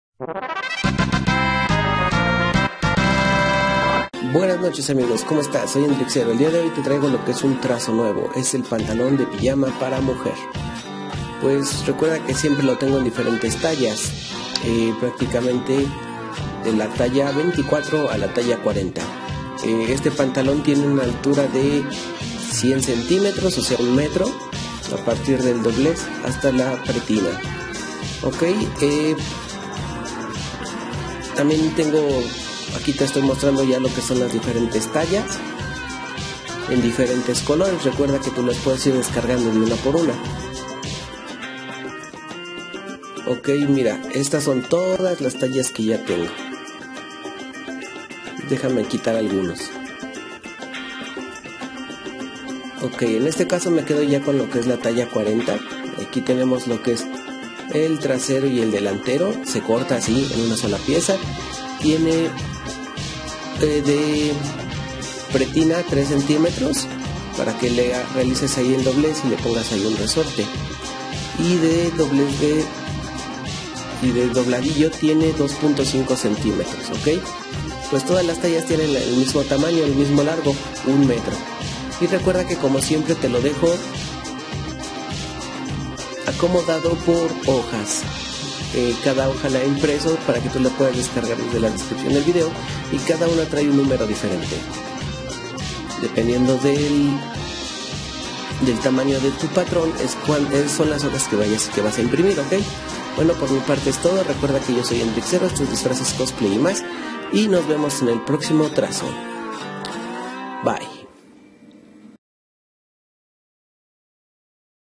Patronaje Pantalón pijama para mujer. Free Patterns pajama pants for woman
Hola amigos, les traigo el trazo para que te hagas tu propio pantalón pijama de la talla 24 a la talla 40.
Talla 24-4 (Contorno cintura 60 cm y Contorno cadera 83 cm.):
Talla 26-6 (Contorno cintura 61 cm y Contorno cadera 88 cm.):
Talla 28-8 (Contorno cintura 63 cm y Contorno cadera 93 cm.)
Talla 30-10 (Contorno cintura 66 cm y Contorno cadera 98 cm.)
Talla 32-12 (Contorno cintura 71 cm y Contorno cadera 102 cm.)
Talla 34-14 (Contorno cintura 76 cm y Contorno cadera 105 cm.)
Talla 36-16 (Contorno cintura 81 cm y Contorno cadera 109 cm.)
Talla 38-18 (Contorno cintura 86 cm y Contorno cadera 112 cm.)
Talla 40-20 (Contorno cintura 91  cm y Contorno cadera 116 cm.)